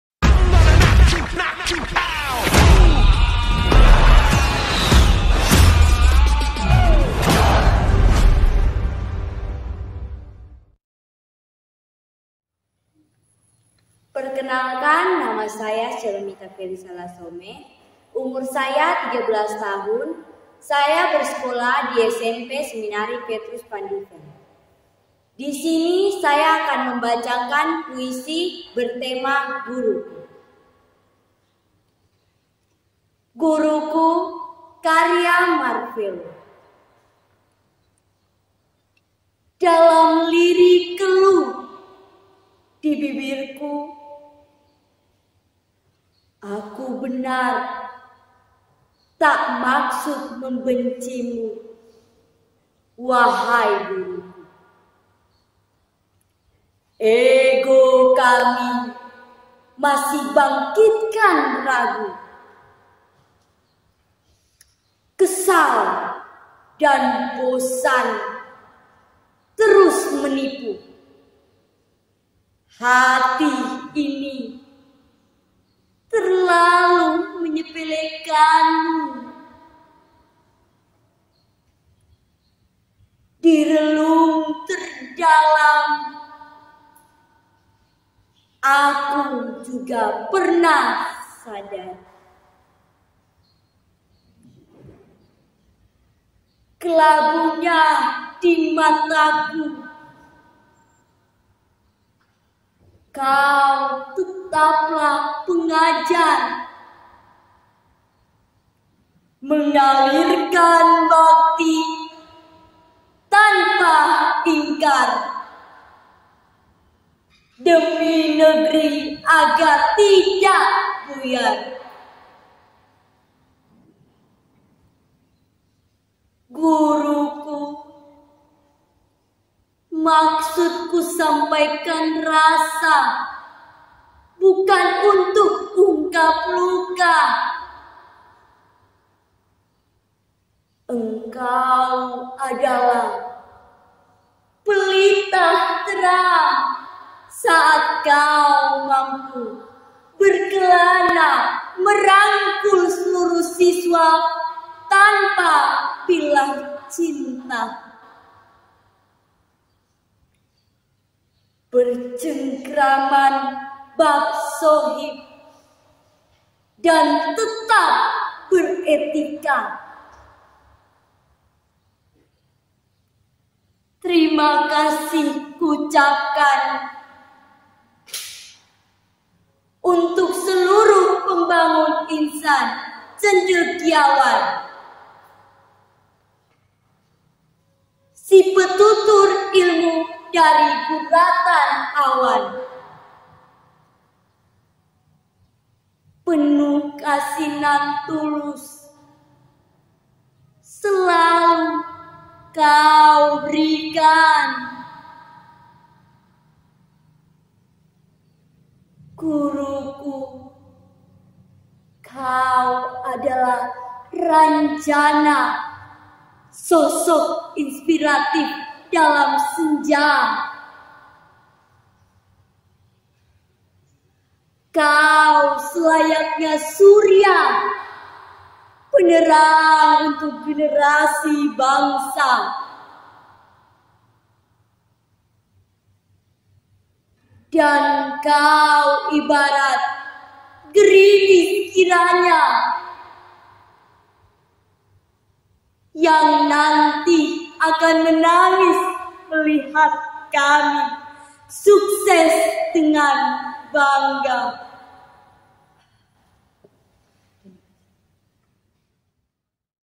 Puisi: GURUKU. Dibacakan oleh Syeilomitha. Siswi SMP Seminari Petrus van Diepen
teks Puisi